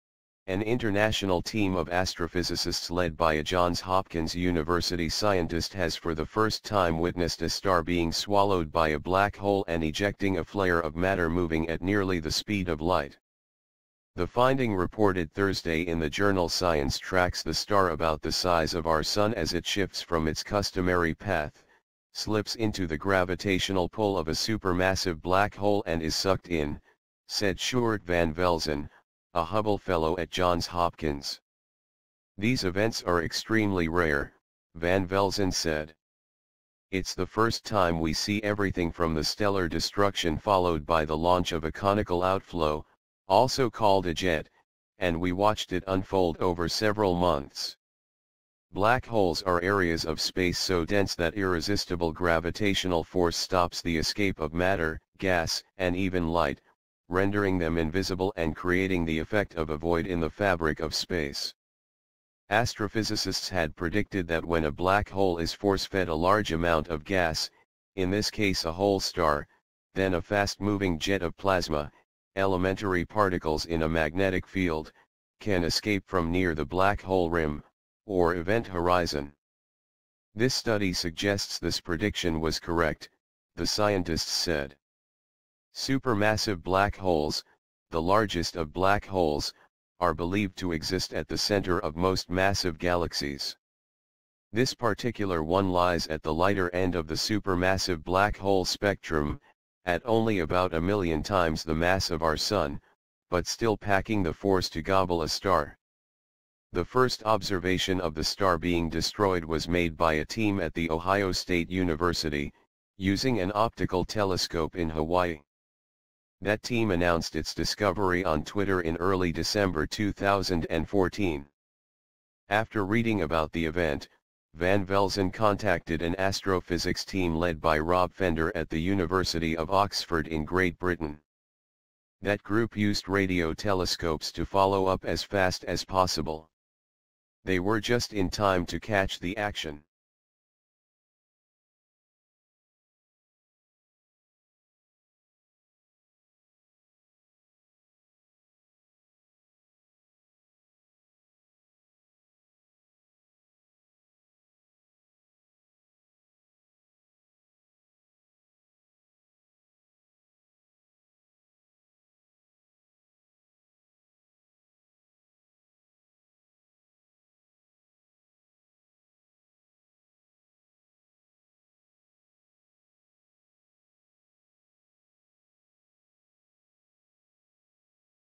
Scientists get first glimpse of black hole eating star, ejecting high-speed flare
An international team of astrophysicists led by a Johns Hopkins University scientist has for the first time witnessed a star being swallowed by a black hole and ejecting a flare of matter moving at nearly the speed of light.